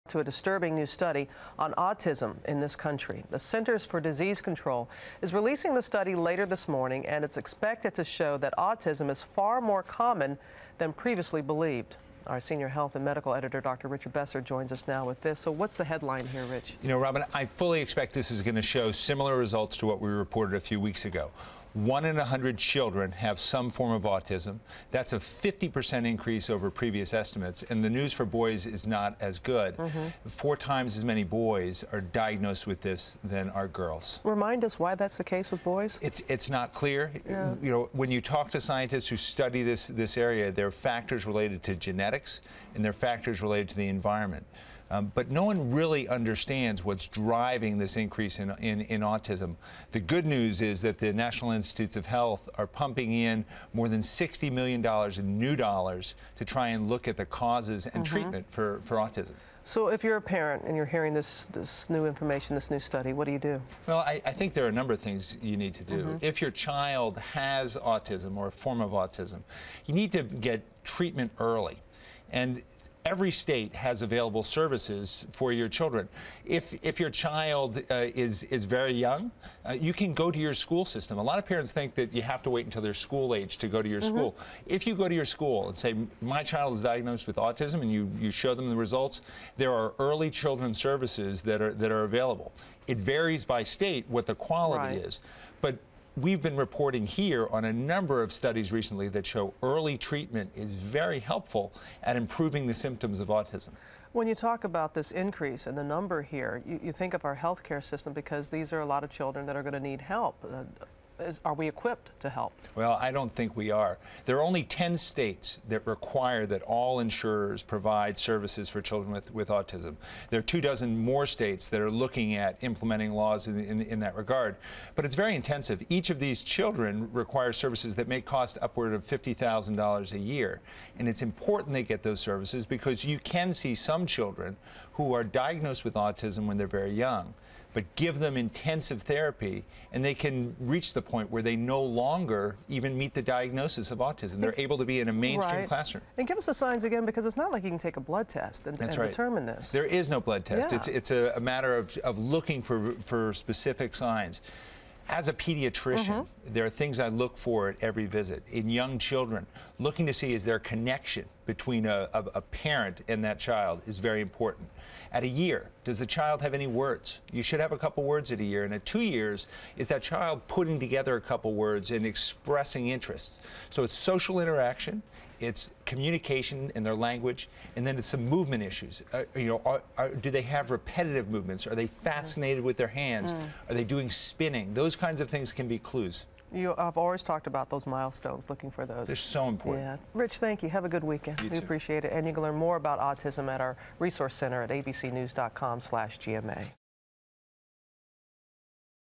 Autism on the Rise
CDC study shows the disease is more common than previously believed.